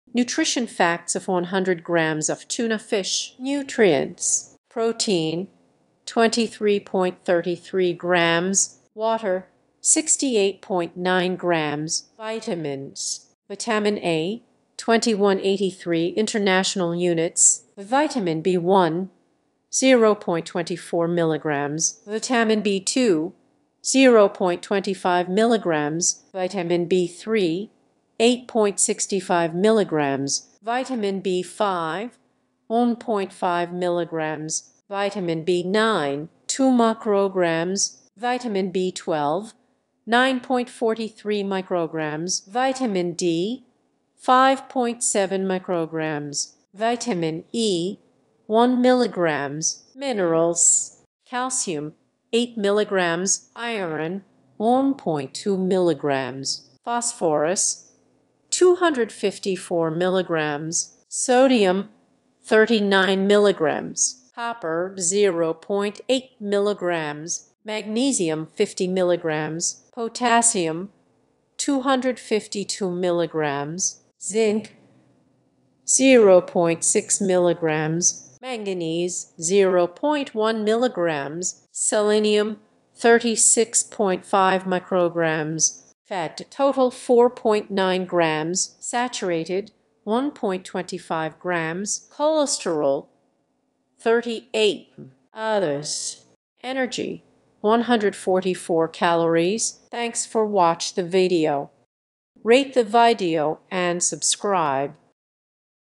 Nutrition Facts of TUNA FISH
Nutrition facts of 100 grams of tuna fish

This video contains technical and precise information about all nutrients, vitamins and minerals of this food.